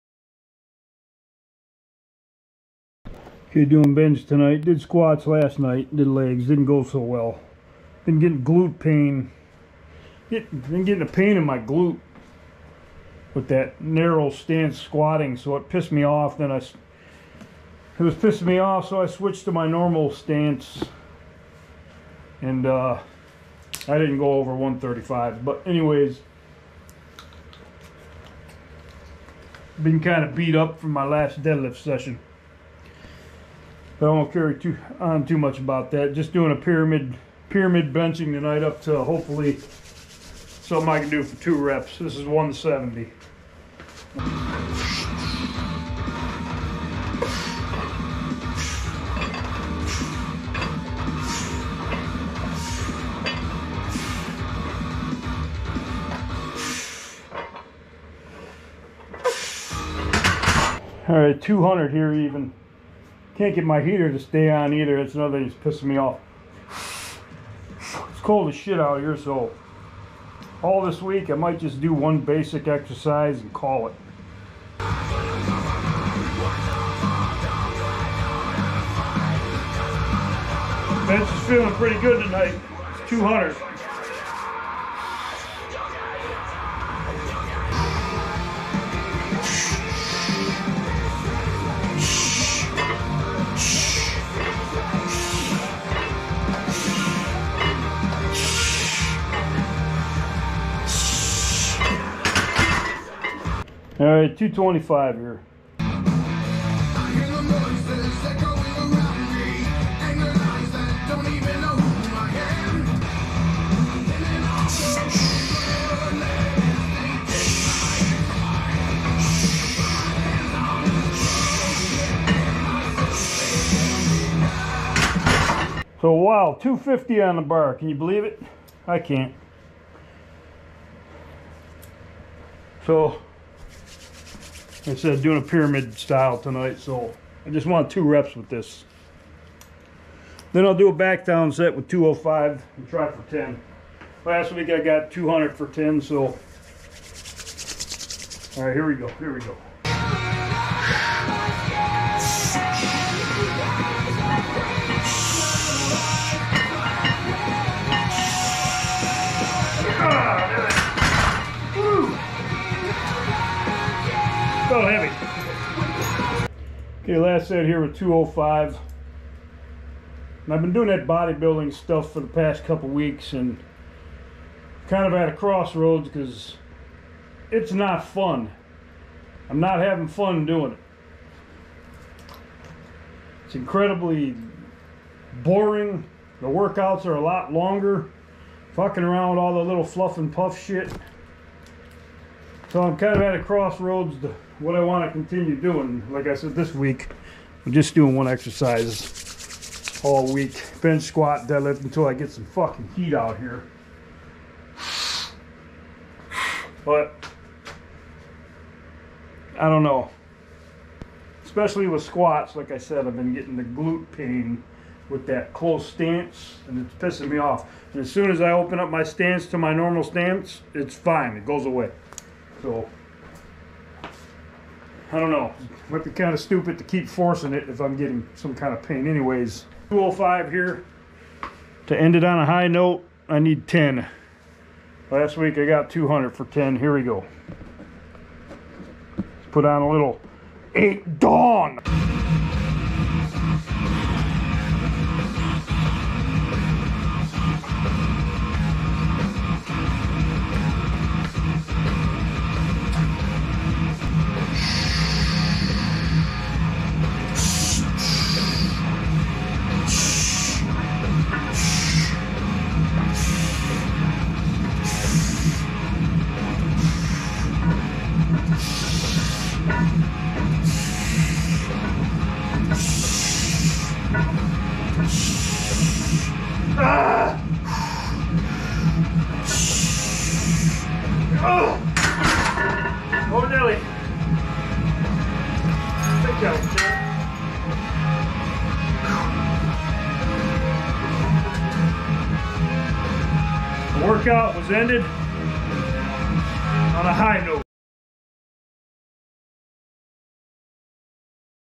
Garage Workouts - Bench Press Pyramid Up to 250lbs with Back Down Set
GWTL
Bench Press
45 x 50
95 x 20
135 x 10
170 x 8
200 x 6
225 x 4
250 x 2
205 x 10